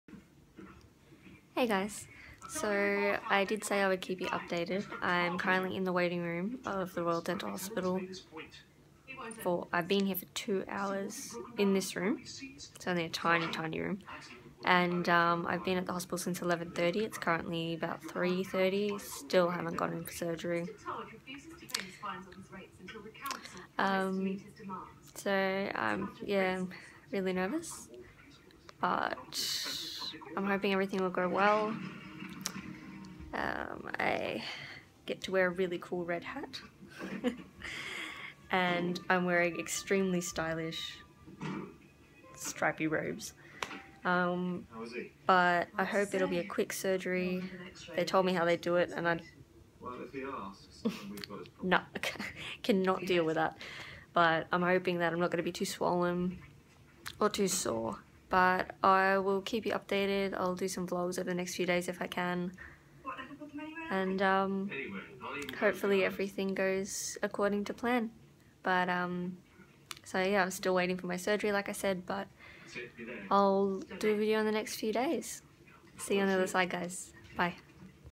Waiting for surgery!! -quick update- So neverous!!
A quick update on my surgery! Don't forget to thumbs up guys and help each other out!